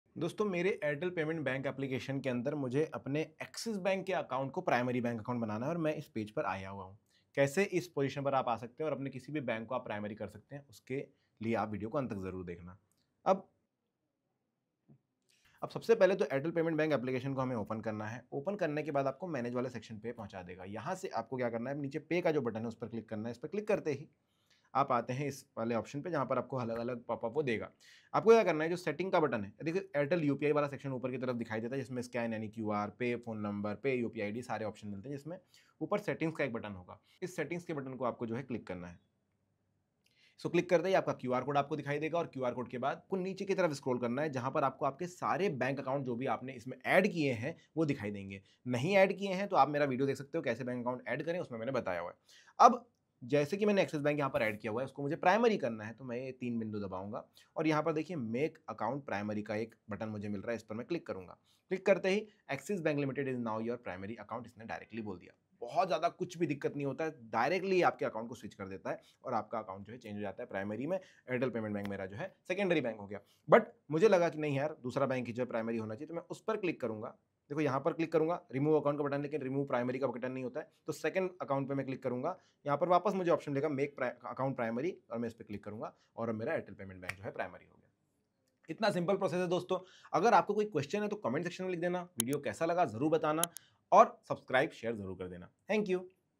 Airtel Payments Bank me Primary Account Kaise Change Kare?
Airtel Payments Bank me Primary Account Kaise Change Kare? Watch this video till the end to learn how to change airtel payment bank primary account. It’s easy and simple to change the primary account of the Airtel payments bank app. Just follow the steps given in the video and it's done.